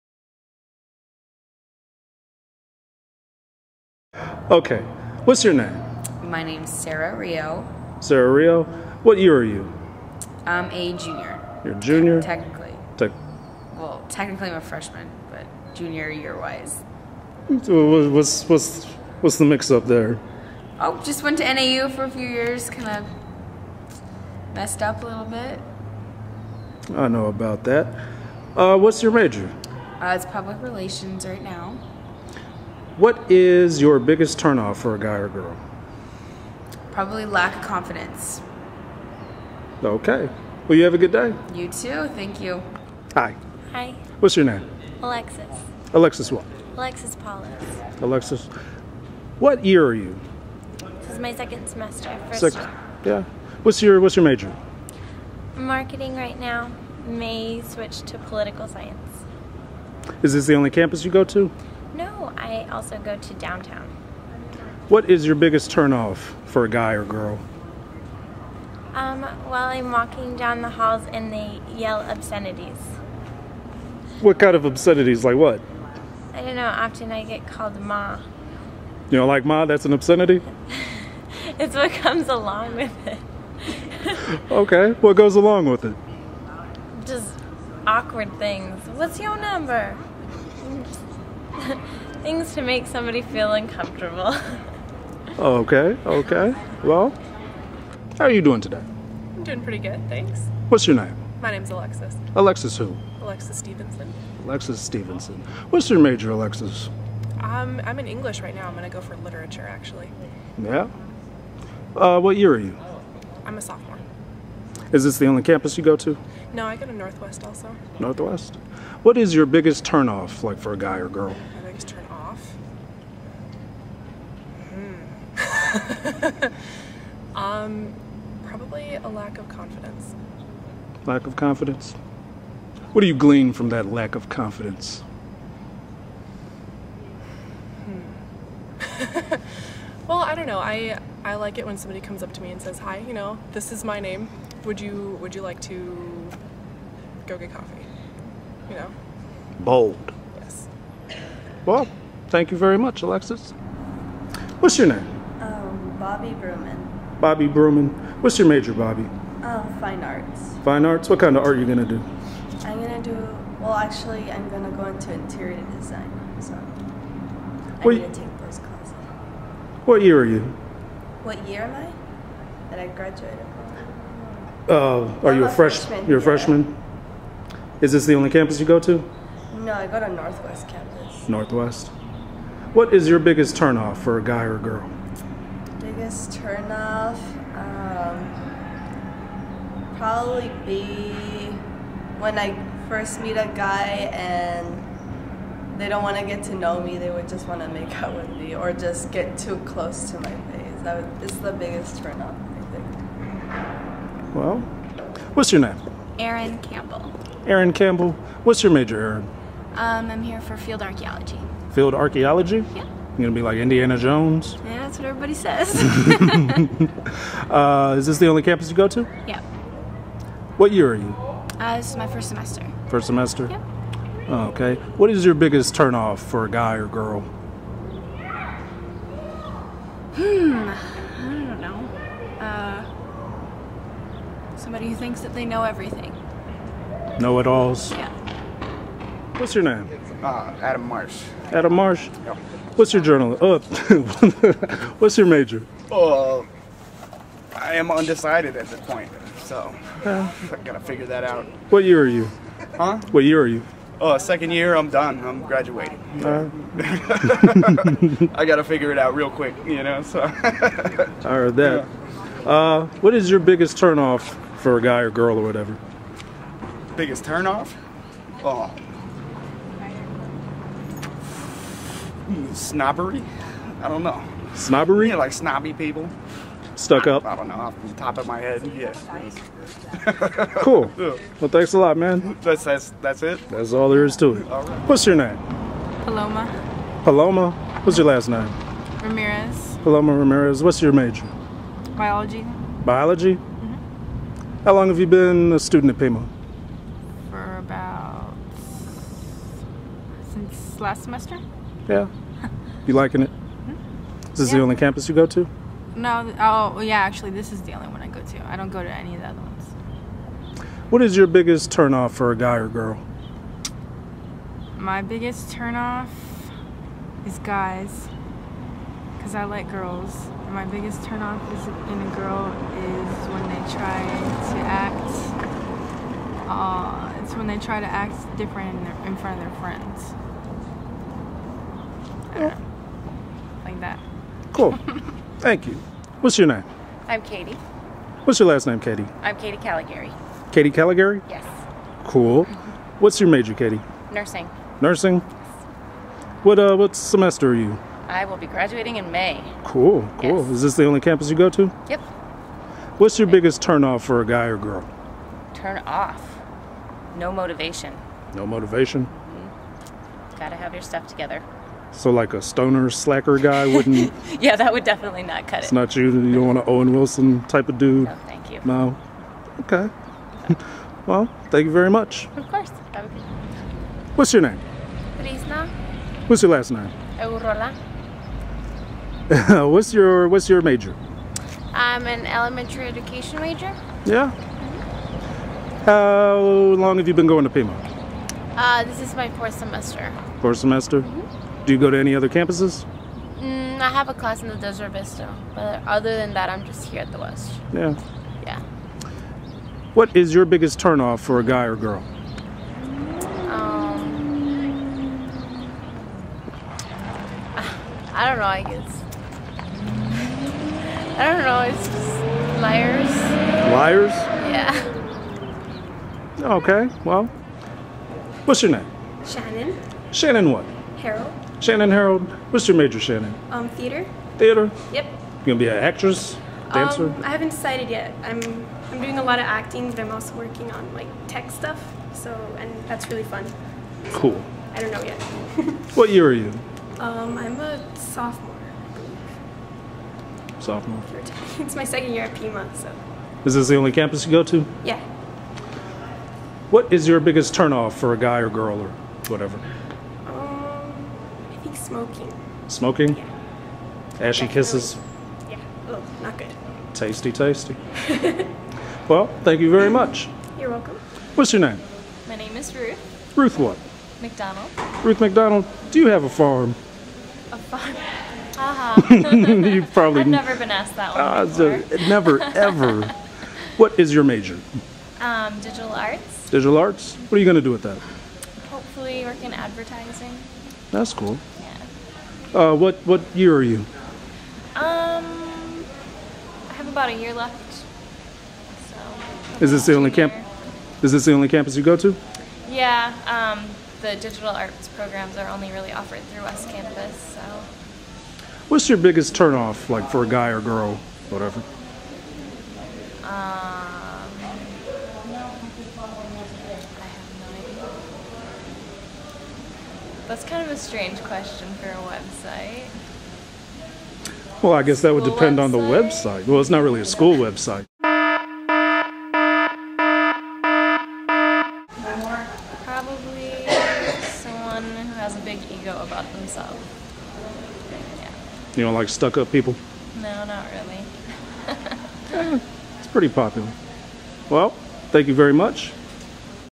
The Word Vol. 61 Issue 3 West Campus
The Aztec Press asks on the Pima Community College West Campus, what is your biggest turn-off?